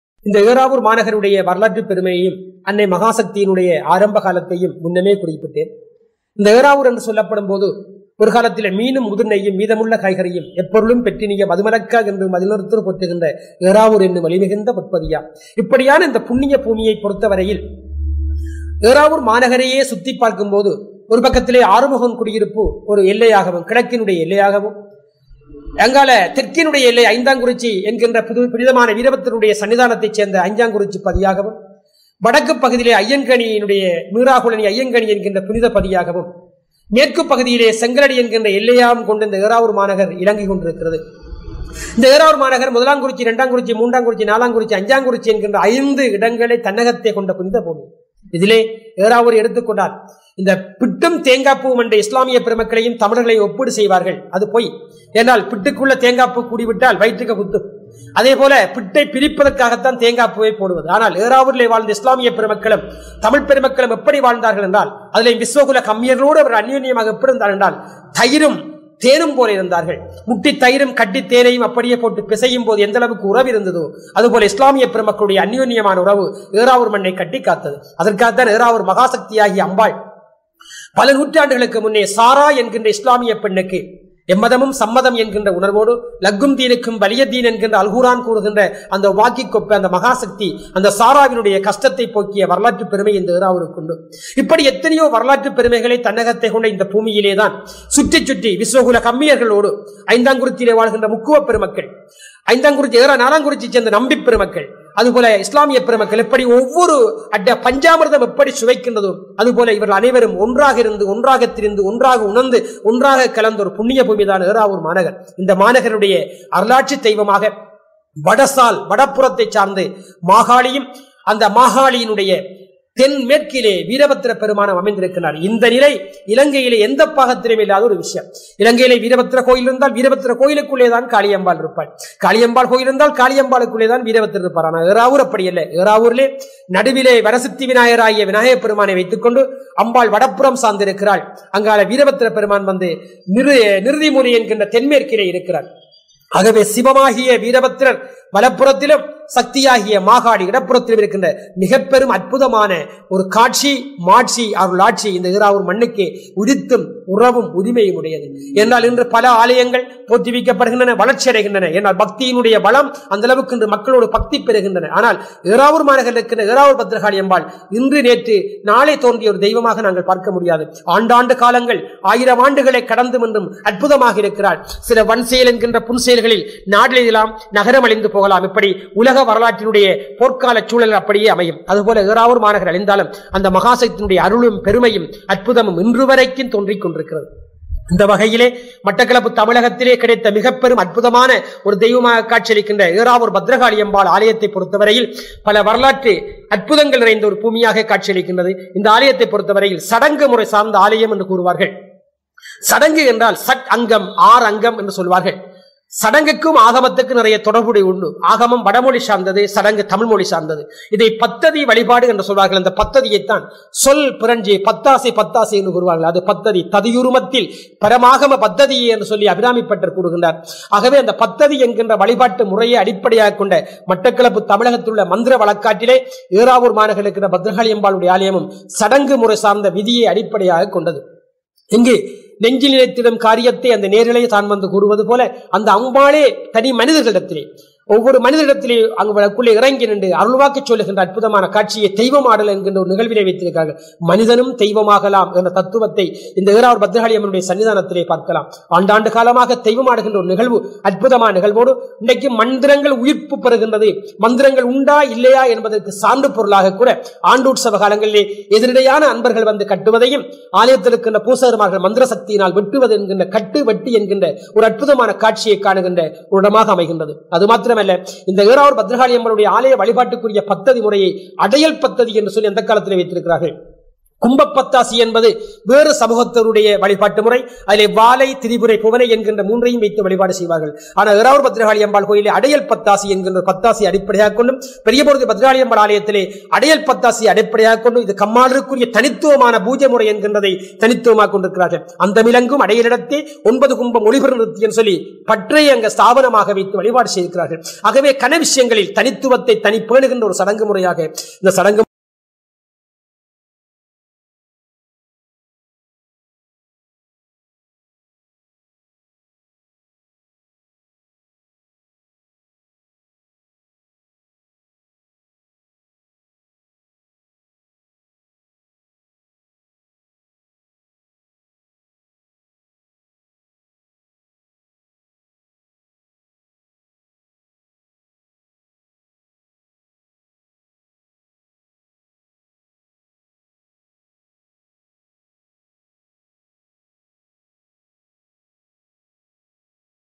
02 ஏறாவூர் ஸ்ரீ பத்திரகாளி அம்பாள்ஆலய வரலாறு பற்றிவிஸ்வப்பிரமம் வை.இ.எஸ்.காந்தன் குருக்கள் அவர்கள்
ஏறாவூர் 04 ஸ்ரீ பத்திரகாளி அம்பாள்ஆலய வரலாறு பற்றிவிஸ்வப்பிரமம் வை.இ.எஸ்.காந்தன் குருக்கள் அவர்கள்